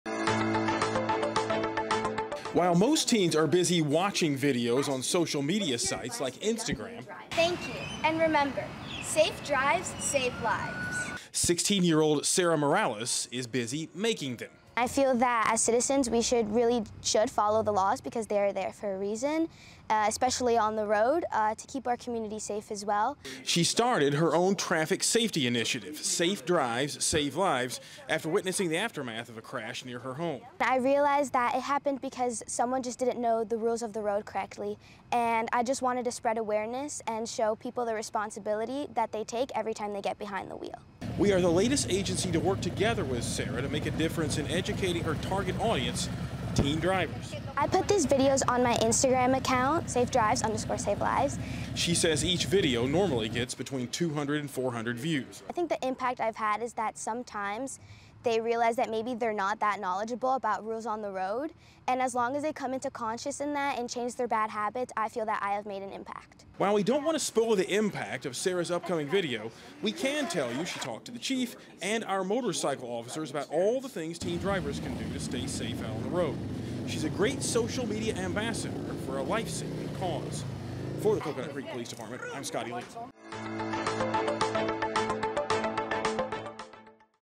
Teen Social Media Phenom Focuses on Teen Driving Safety
While most teens are busy watching videos on social media sites like Instagram, Sarah Morales in making them. We're working together with her to make a difference in traffic safety for teen drivers. Her Instagram page, @SafeDrives_SaveLives is focused on partnering with local police departments to get safety tips to teen drivers.